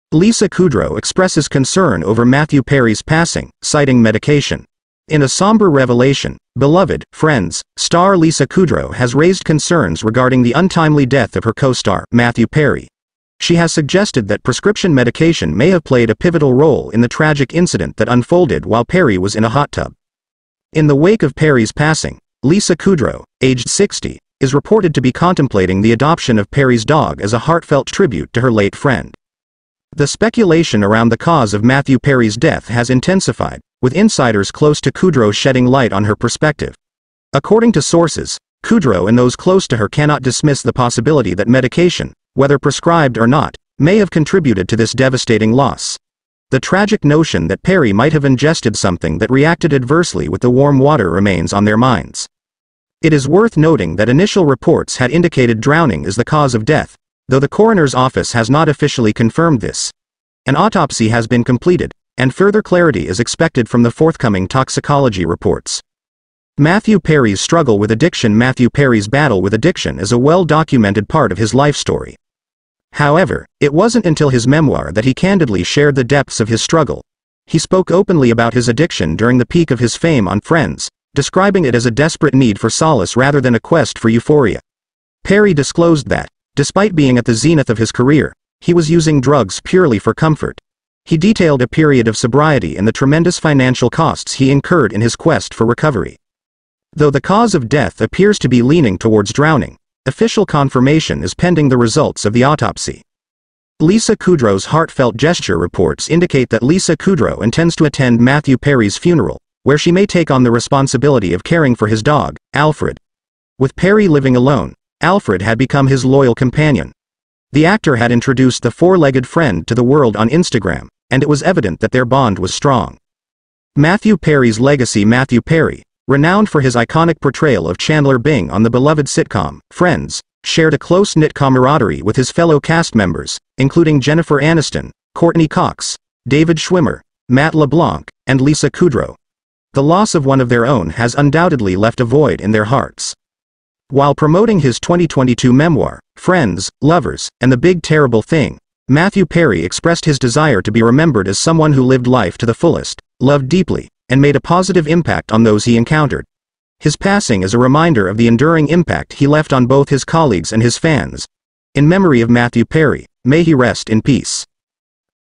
Lisa Kudrow Prescription Medication Played A Role In Matthew Perry’s Death
Lisa Kudrow Expresses Concern Over Matthew Perry's Passing, Citing Medication

In a somber revelation, beloved "Friends" star Lisa Kudrow has raised concerns regarding the untimely death of her co-star, Matthew Perry. She has suggested that prescription medication may have played a pivotal role in the tragic incident that unfolded while Perry was in a hot tub.

In the wake of Perry's passing, Lisa Kudrow, aged 60, is reported to be contemplating the adoption of Perry's dog as a heartfelt tribute to her late friend.

The speculation around the cause of Matthew Perry's death has intensified, with insiders close to Kudrow shedding light on her perspective. According to sources, Kudrow and those close to her cannot dismiss the possibility that medication, whether prescribed or not, may have contributed to this devastating loss. The tragic notion that Perry might have ingested something that reacted adversely with the warm water remains on their minds.

It is worth noting that initial reports had indicated drowning as the cause of death, though the coroner's office has not officially confirmed this. An autopsy has been completed, and further clarity is expected from the forthcoming toxicology reports.

Matthew Perry's Struggle with Addiction

Matthew Perry's battle with addiction is a well-documented part of his life story. However, it wasn't until his memoir that he candidly shared the depths of his struggle. He spoke openly about his addiction during the peak of his fame on "Friends," describing it as a desperate need for solace rather than a quest for euphoria. Perry disclosed that, despite being at the zenith of his career, he was using drugs purely for comfort. He detailed a period of sobriety and the tremendous financial costs he incurred in his quest for recovery.

Though the cause of death appears to be leaning towards drowning, official confirmation is pending the results of the autopsy.

Lisa Kudrow's Heartfelt Gesture

Reports indicate that Lisa Kudrow intends to attend Matthew Perry's funeral, where she may take on the responsibility of caring for his dog, Alfred. With Perry living alone, Alfred had become his loyal companion. The actor had introduced the four-legged friend to the world on Instagram, and it was evident that their bond was strong.

Matthew Perry's Legacy

Matthew Perry, renowned for his iconic portrayal of Chandler Bing on the beloved sitcom "Friends," shared a close-knit camaraderie with his fellow cast members, including Jennifer Aniston, Courteney Cox, David Schwimmer, Matt LeBlanc, and Lisa Kudrow. The loss of one of their own has undoubtedly left a void in their hearts.

While promoting his 2022 memoir, "Friends, Lovers, and the Big Terrible Thing," Matthew Perry expressed his desire to be remembered as someone who lived life to the fullest, loved deeply, and made a positive impact on those he encountered. His passing is a reminder of the enduring impact he left on both his colleagues and his fans.

In memory of Matthew Perry, may he rest in peace.